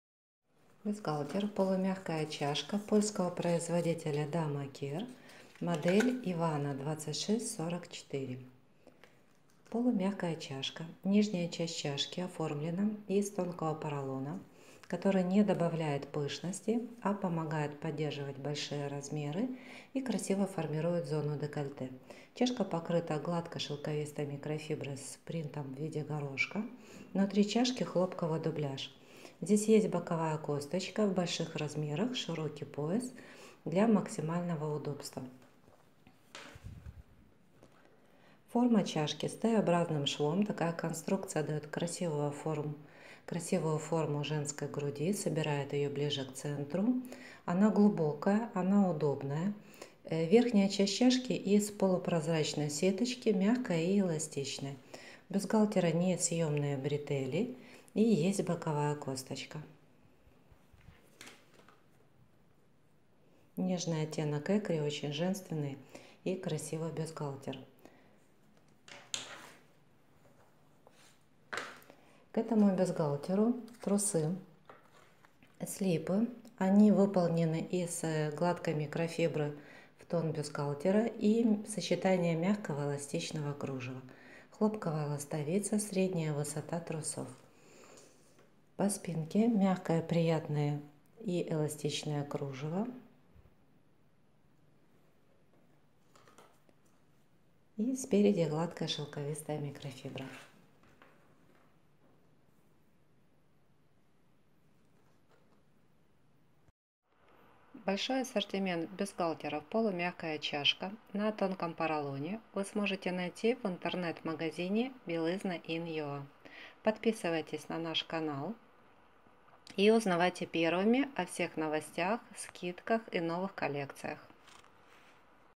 Бюстгальтер полумягкий Dama Kier Ivana Экри Bilizna.in.ua
Dama Kier Ivana  бюстгальтер для пышной груди.

Женственный и элегантный дизайн в светлом оттенке экри.

Романтичный принт « горошек».

Полумягкая чашка помогает поддерживать тяжелую грудь,

красиво формируя зону декольте.

Бретели несъемные. Внутри хлопковый дубляж.

Dama Kier красивое  польское нижнее бельё.
Подробнее о цене и размерах по ссылке